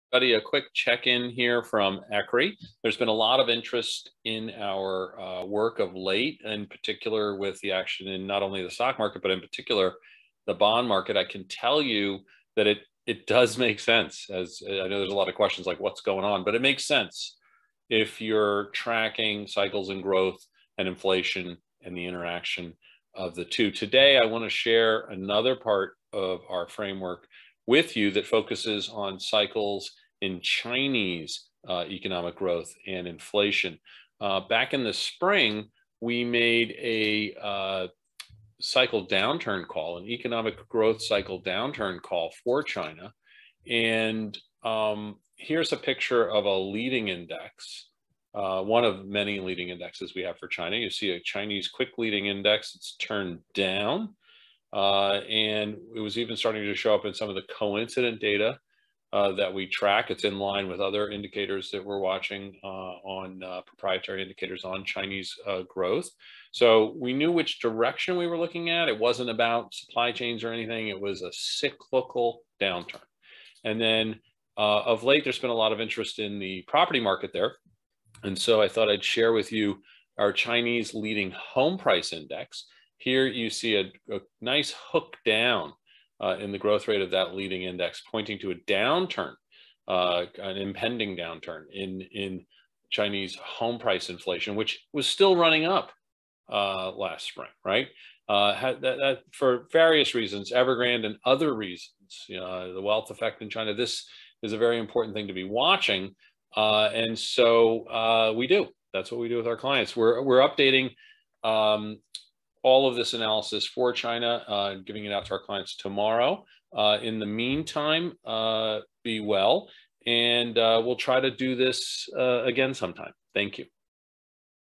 ECRI: Checking in on China (November 9, 2021)
ECRI's Lakshman Achuthan shares an update in relation to recent events in Chinese growth and inflation, including details on housing.